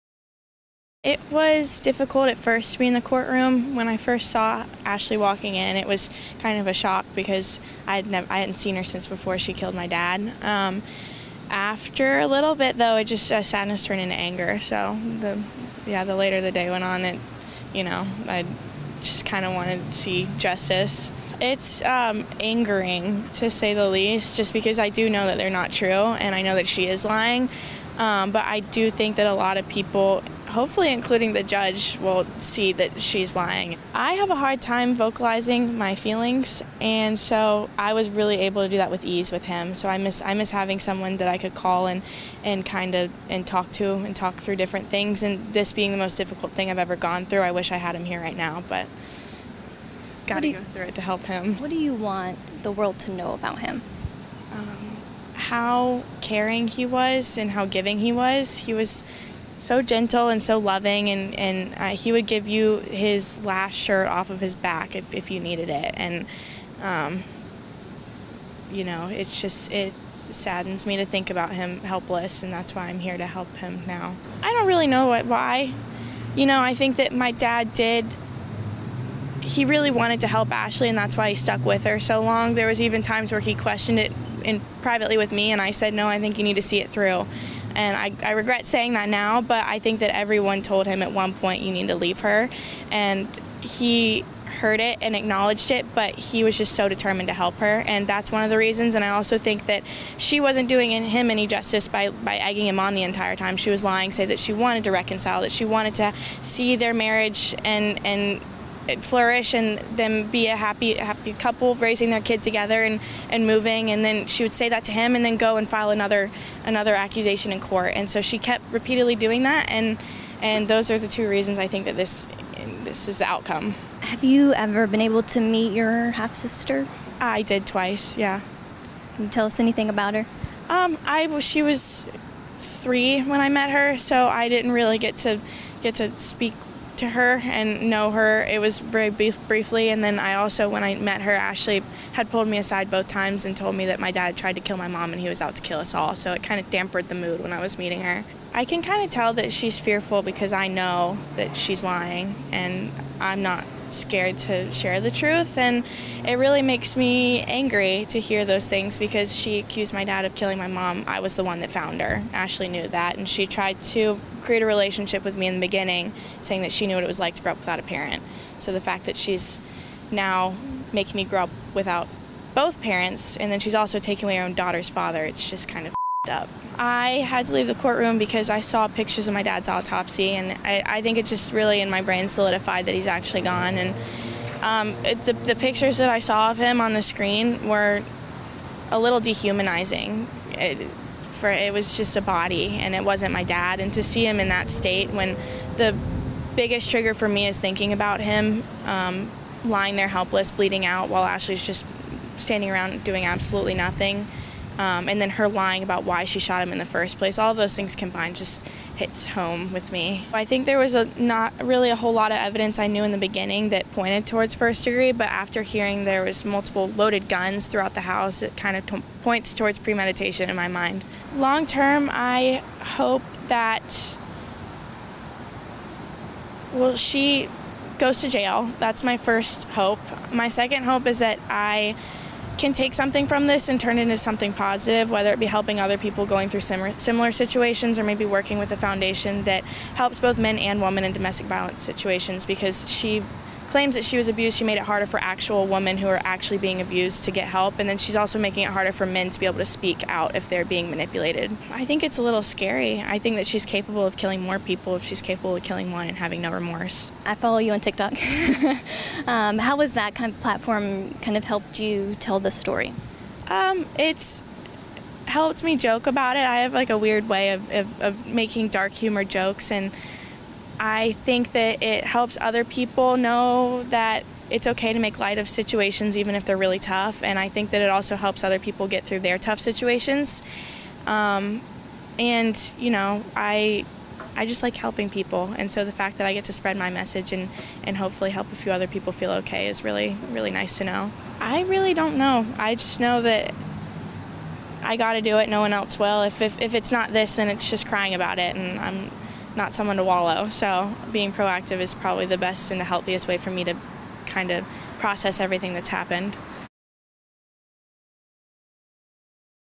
Eva Benefield Reacts to Stepmother’s Hearing
“Sadness turned into anger.” The victim's daughter Eva Benefield told CourtTV's Chanley Painter outside former ballerina AshleyBenefield’s ‘Stand Your Ground’ hearing in Florida on July 6, 2023.

Ashley Benefield charged in her husband’s death was in court last week to argue she acted in self-defense.